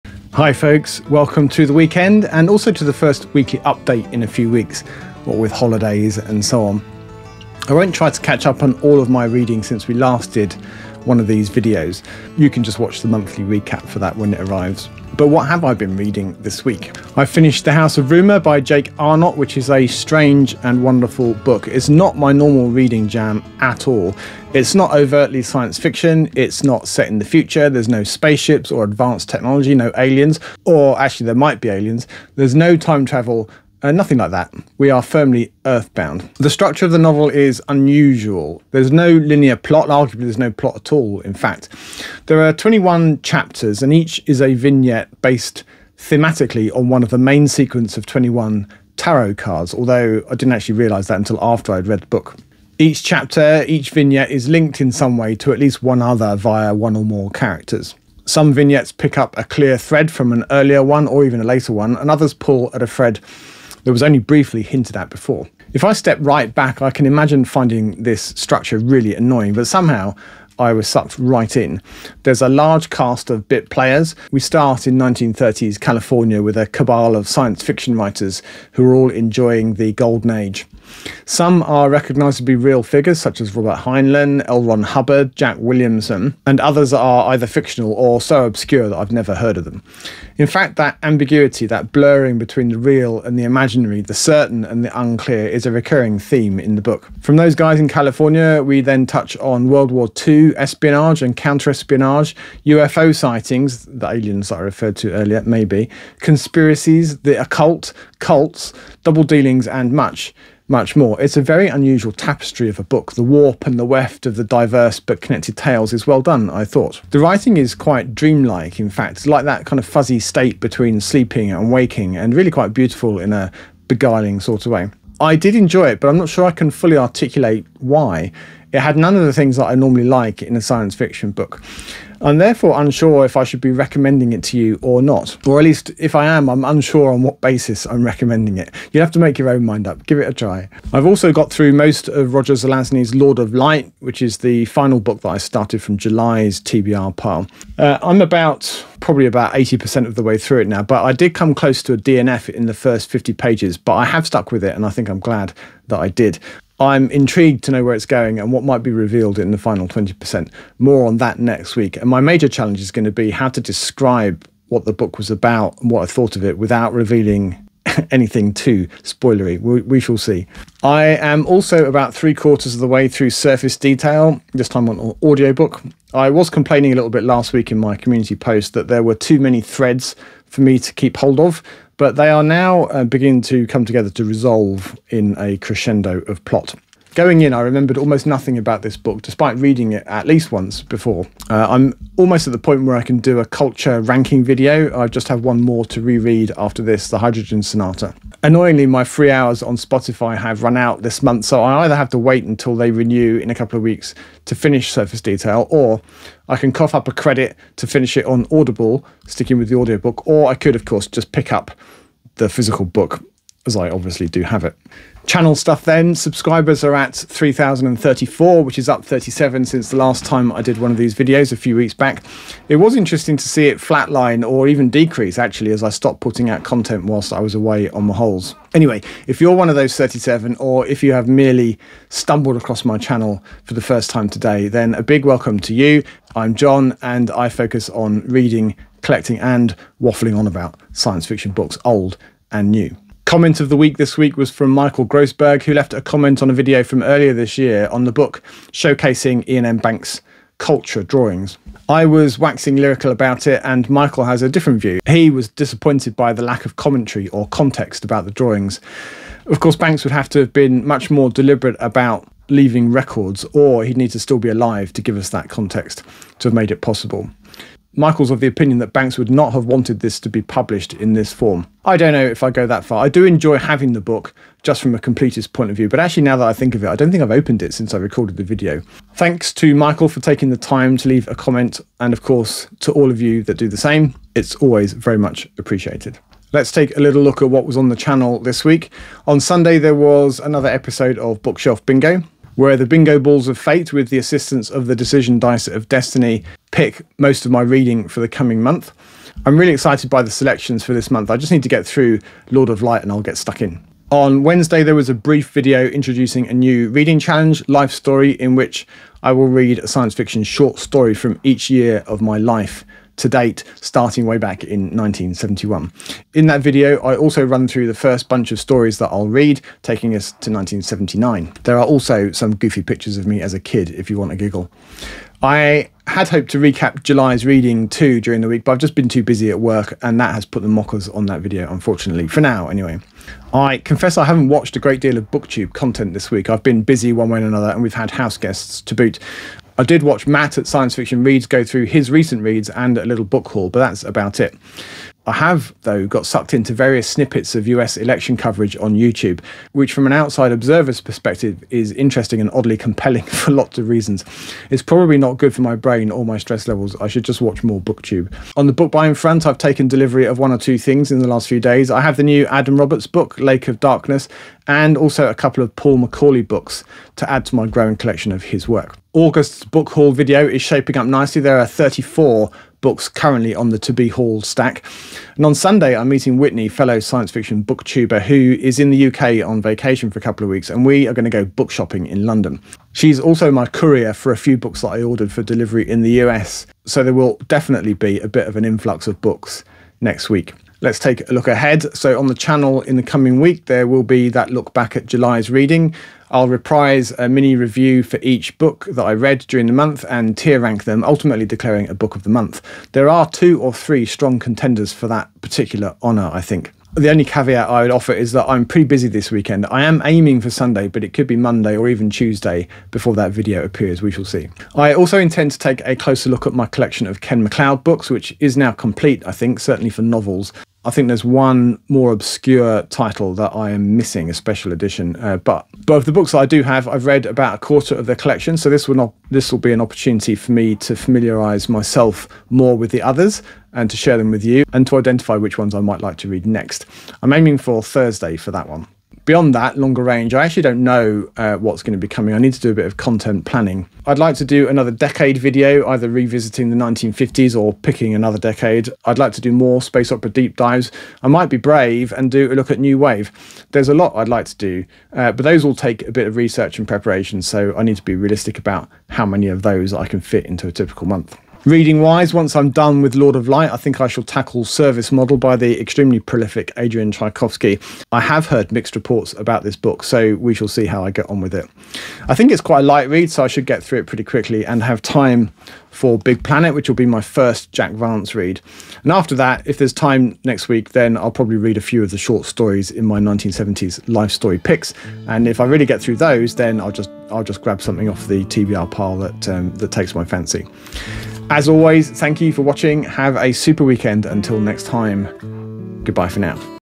Weekly Sci Fi Update | 2nd August 2024
My week in science fiction!  What I read during the week, comment of the week, a book buying bulletin, a look ahead, what I watched and much more.

In the unlikely event you want to buy me a book!  Amazon Wishlist: